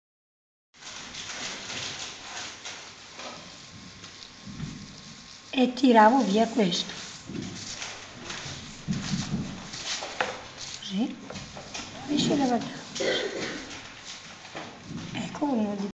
Come le nonne pulivano i bachi
Gli alunni 3T delle classi 4^A 4^B della Scuola Primaria di Tezze IC2- ARZIGNANO(VI)  con le inss. Rosa Maria Vicariotto e Nerina Carlotto
a.s.2013-14